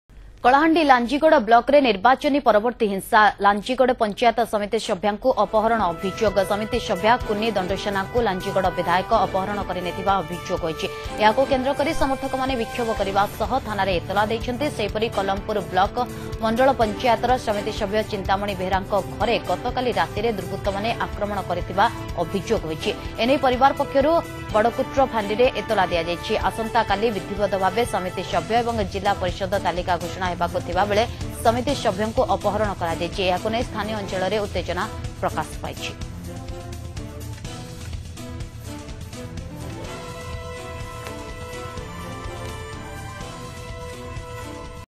Opponents kidnap PS Member in Kalahandi
In yet another poll-related incident, an elected Panchayat Samiti Member has allegedly been kidnapped by supports of his defeated rival in Odisha’s Kalahandi district.